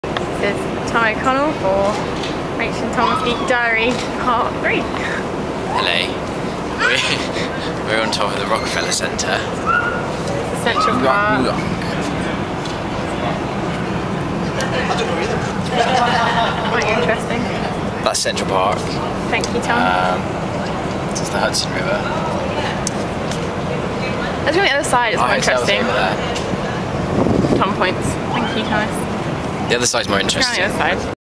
Tom and Rachel's New York Adventures...Part 4 - On Top of the Rock
Tom guides us round the skyscrapers of New York City from the Top of the Rockerfeller Centre